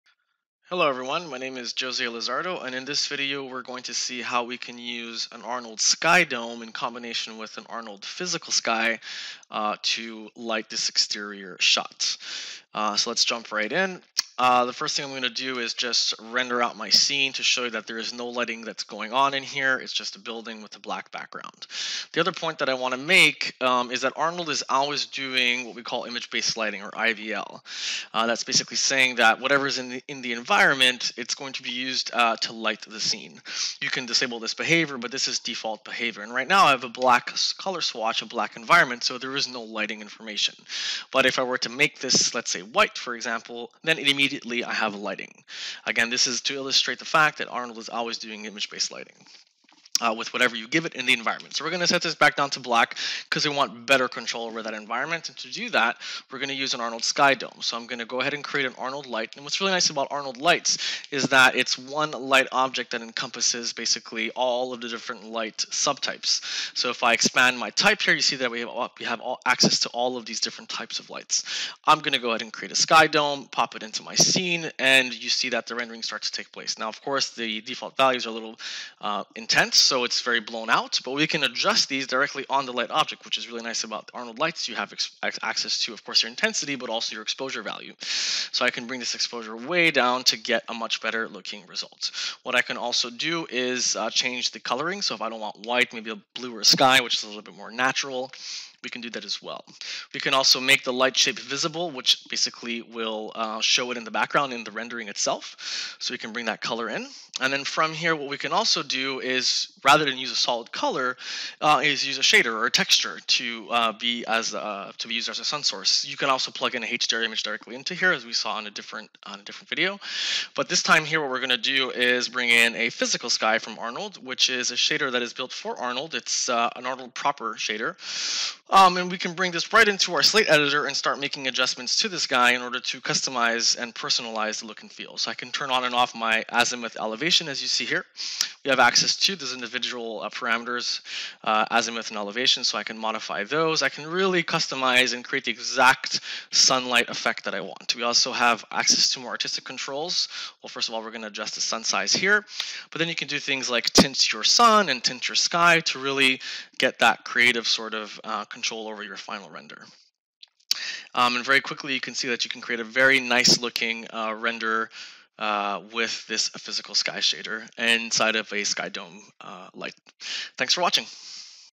3ds max & Arnold Exterior Lighting - Using an Arnold Skydome
In this video we will see how to use an Arnold Skydome to illuminate our Building.